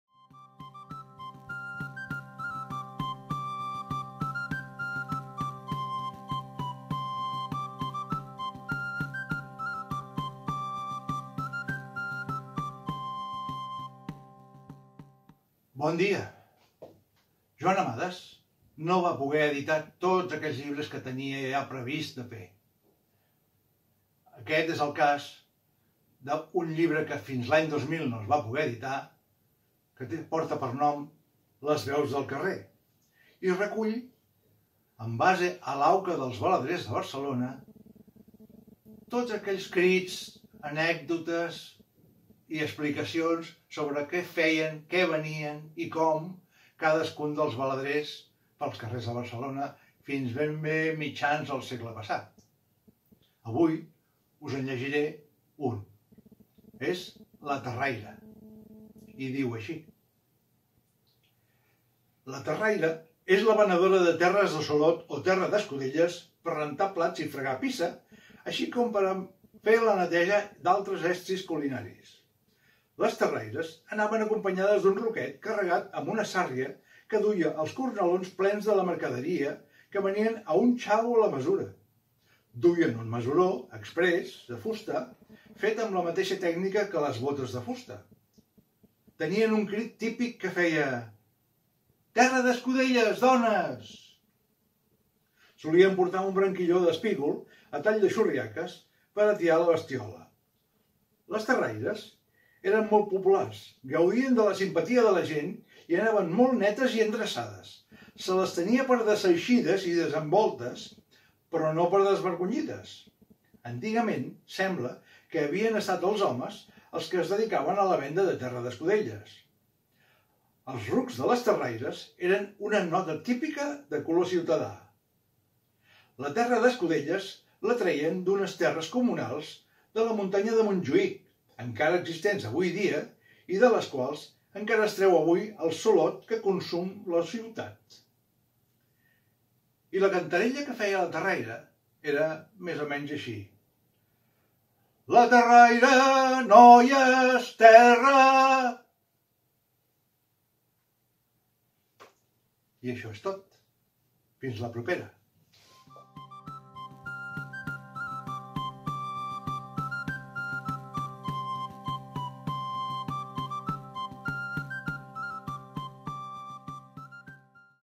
L'Amades, confinat. (Lliurament 35: Els Baladrers de Barcelona)
L’Associació Cultural Joan Amades us ofereix diàriament i durant el període de confinament per el Coronavirus, breus lectures comentades de l’obra de Joan Amades. Desitgem que us agradi.

AC Joan Amades